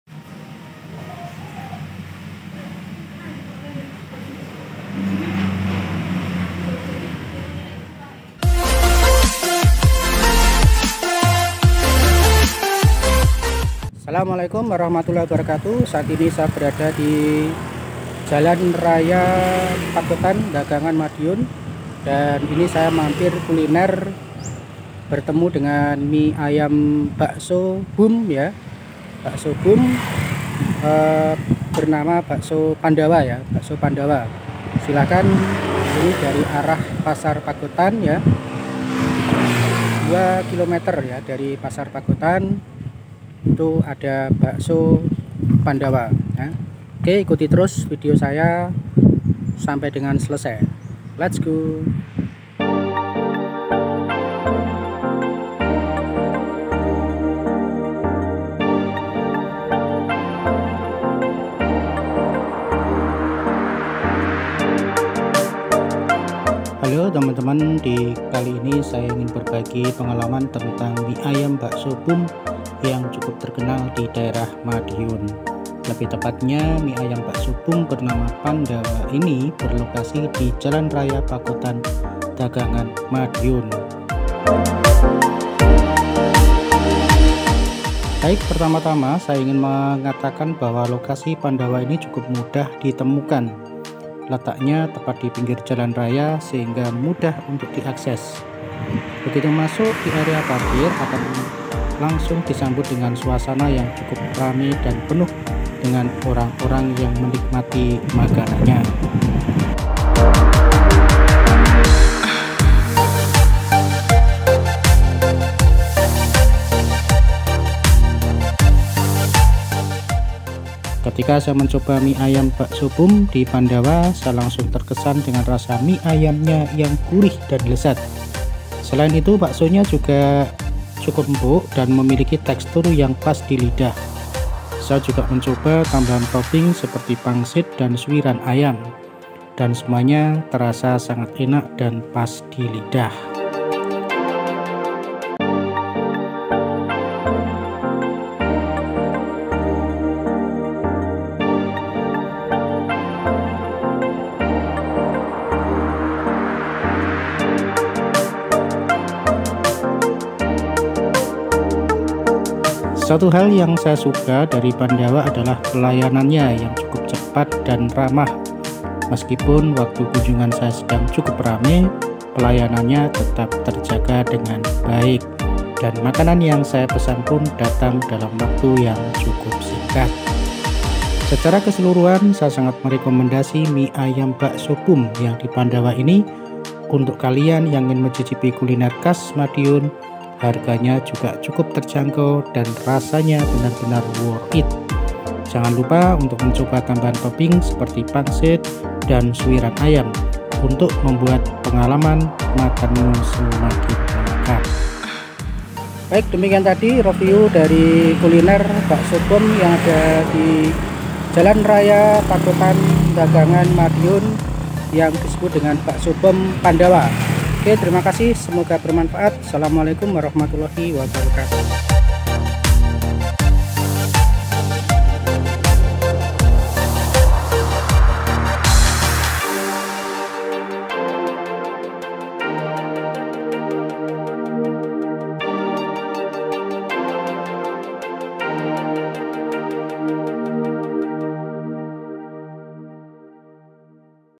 Kelezatan Mie Ayam dan Bakso BOOM di Pagotan Madiun: Cicipi Rasanya di Pandawa
Siapa yang tidak suka mie ayam dan bakso? Di Pagotan Madiun terdapat sebuah warung kuliner legendaris bernama Pandawa yang menyajikan mie ayam dan bakso BOOM dengan rasa yang melegenda. Video ini akan membawa Anda untuk mencicipi kelezatan mie ayam dan bakso BOOM di Pandawa, di mana mie dan baksonya berisi banyak daging dan sangat enak. Selain itu, video ini juga akan menunjukkan suasana warung dan pengalaman makan di sana. Jika Anda sedang berkunjung ke Madiun, jangan lewatkan untuk mencicipi mie ayam dan bakso BOOM terlezat di Pagotan Madiun di Pandawa!